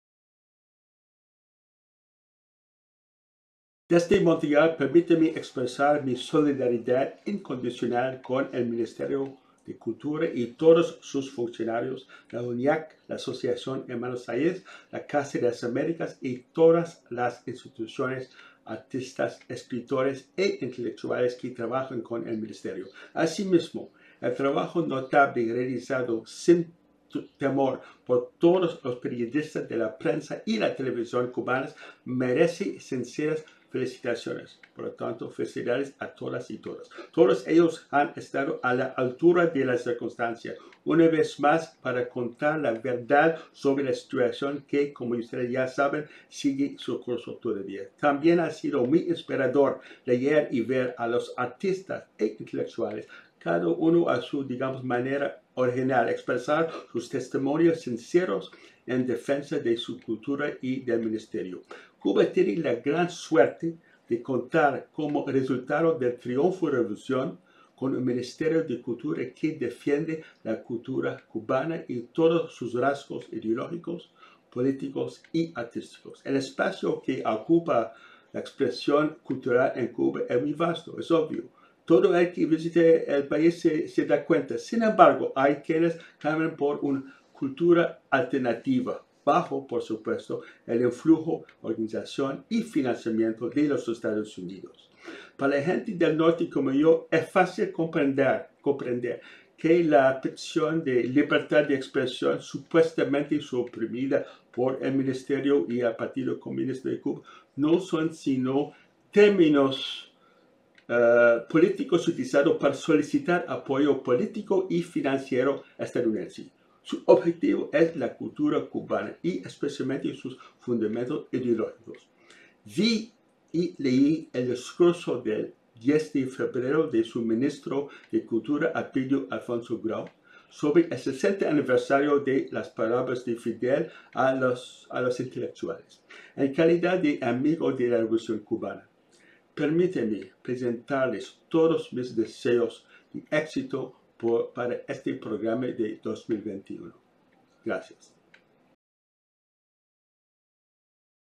PorCuba /Por su Cultura: Arnold August, "solidaridad incondicional con el Ministerio de Cultura"
Mensaje de Arnold August, periodista, escritor canadiense e integrante de la REDH, para la Campaña Por Cuba/Por su Cultura, coordinada por la Red en Defensa de la Humanidad ante el asedio permanente a Cuba y las instituciones de su cultura.